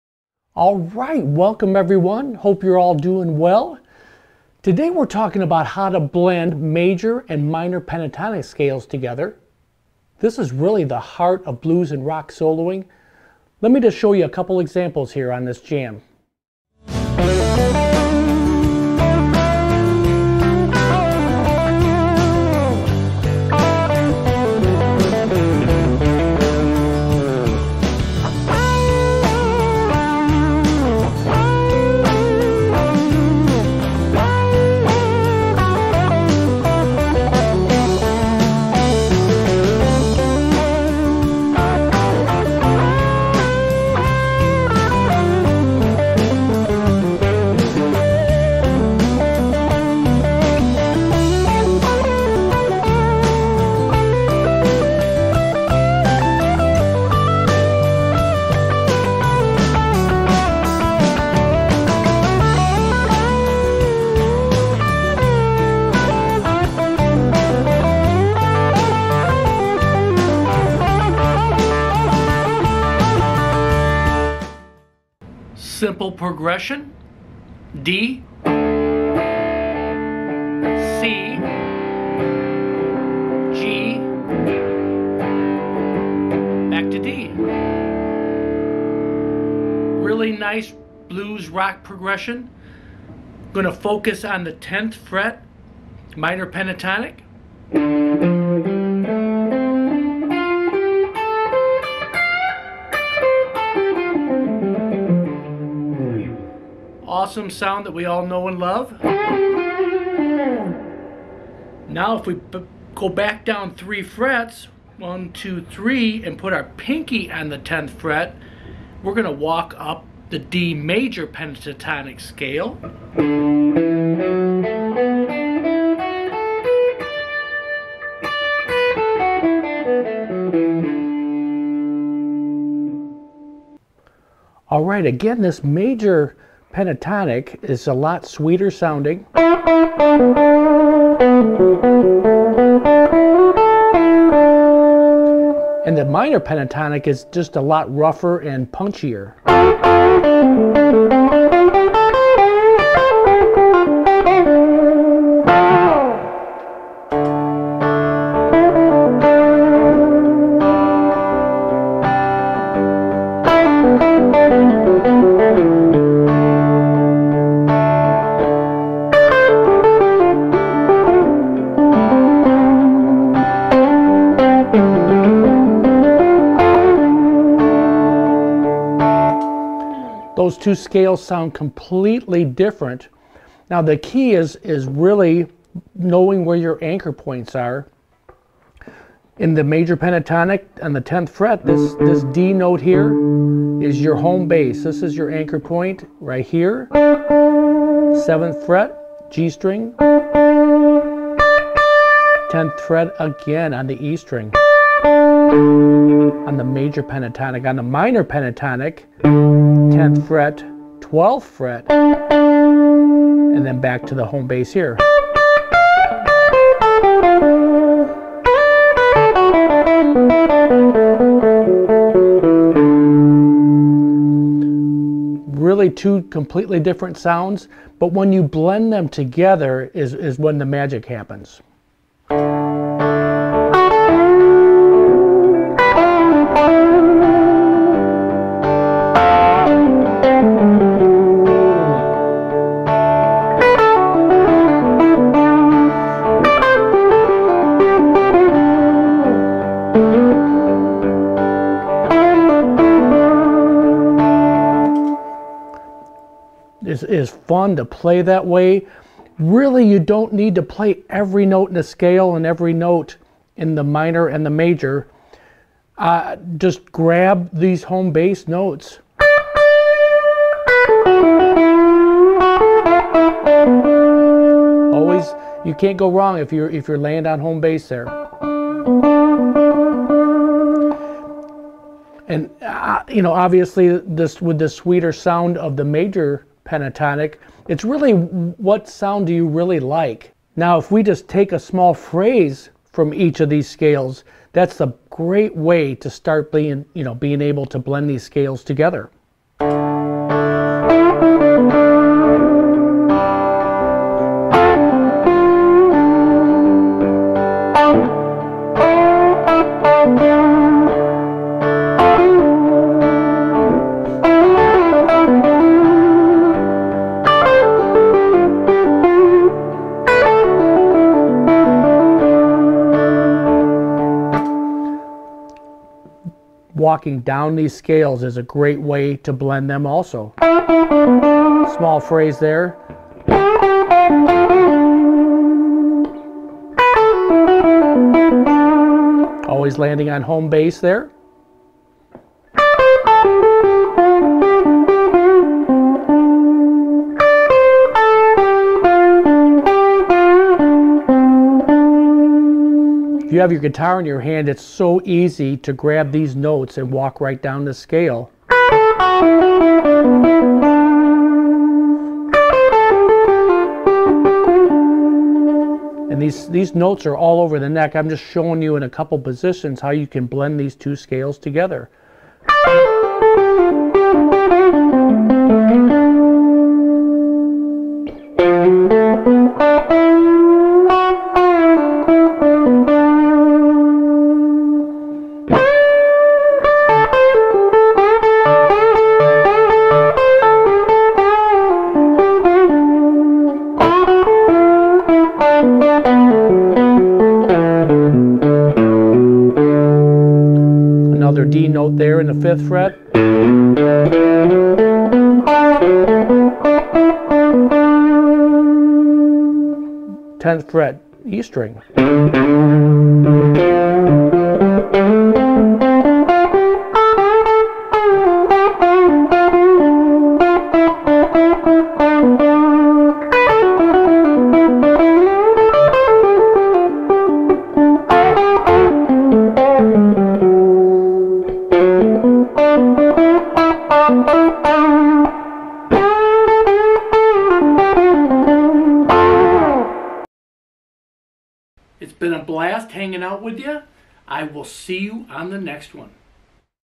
How To Blend Major And Minor Pentatonic Scales
In todays lesson I will show you how to blend the major and minor pentatonic  scales together. We will add major notes to the minor pentatonic scale. This is a easy way blend the 2 scales together. This is a intermediate guitar lesson. For someone who knows chords and the pentatonic scale. Blending the scales together seems hard but check out this technique. Hope you get something from the lesson! Let’s improve our solos together!
-Video Contents-
0:00 Lesson Intro
0:16 Intro Jam
1:15 Major and Minor Pentatonic Scales
2:14 Lesson/Blending the major and minor pentatonic scales